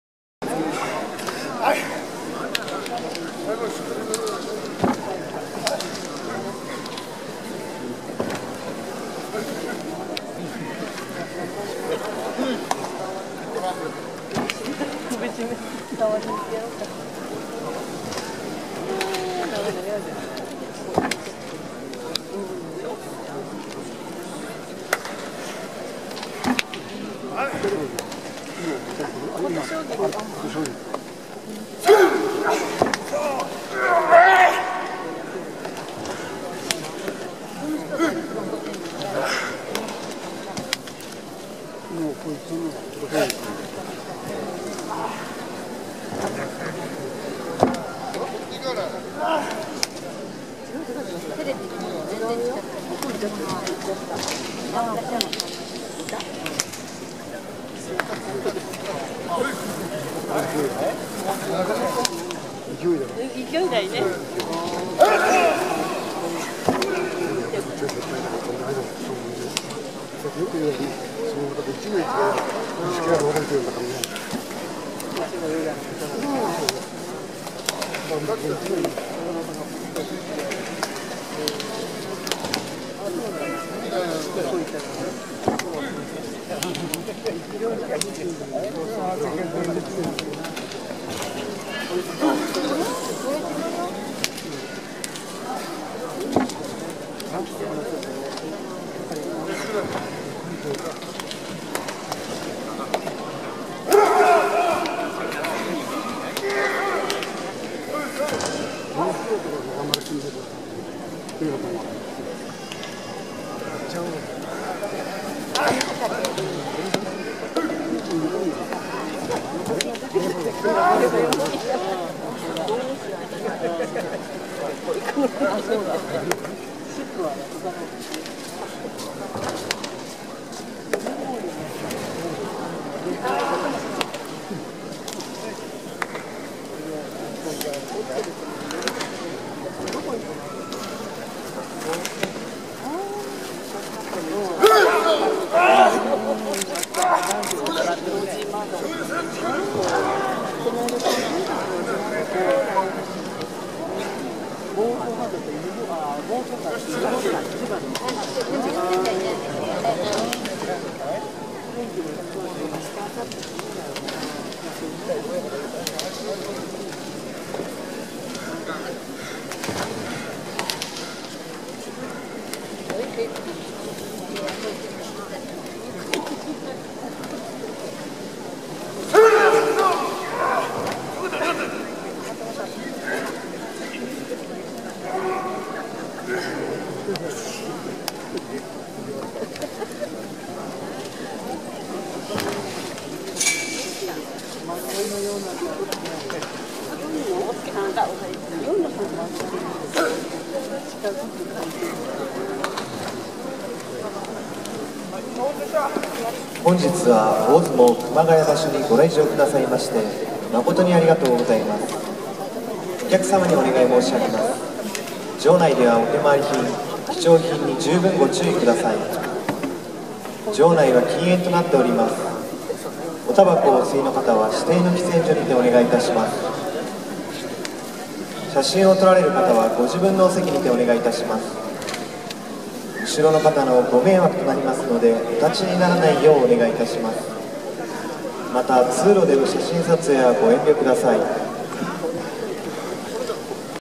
照ノ富士、玉鷲にぶつかり稽古で鍛えられる（平成25年秋･熊谷巡業 SUMO TOUR in Kumagaya 2013）
2013年10月12日(土)、熊谷巡業に行ってきました！

先場所に十両優勝した照ノ富士。
さっそく玉鷲にぶつかり稽古で鍛えられてました。

Grand sumo jyungyo at Sainokuni-kumagaya-dome in Kumagaya.